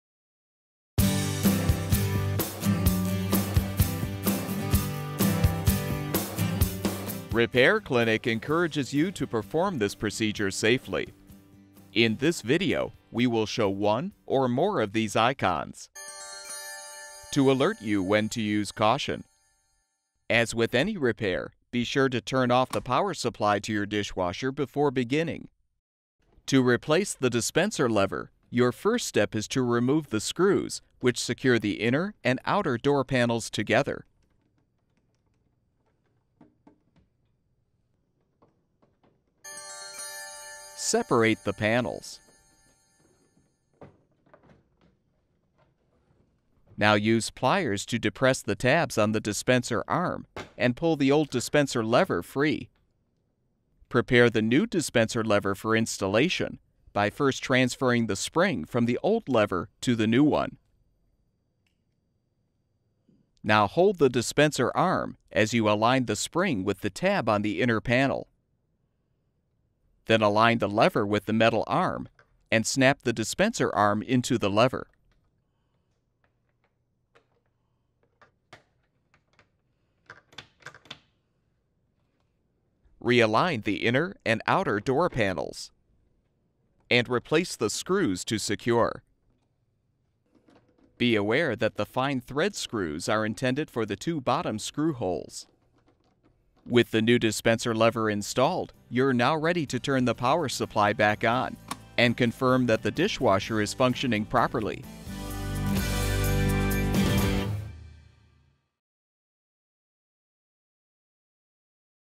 GE Dishwasher Dispenser Lever Replacement
This video provides step-by-step instructions for replacing the dispenser lever on GE dishwashers. The most common reason for replacing the lever is if it is broken, causing the dispenser lid to not open during the cycle.

Additional dishwasher repair, troubleshooting tips, help with finding your model number, and part replacement videos

All of the information in this dispenser lever replacement video is applicable to the following brands: GE, Hotpoint, Kenmore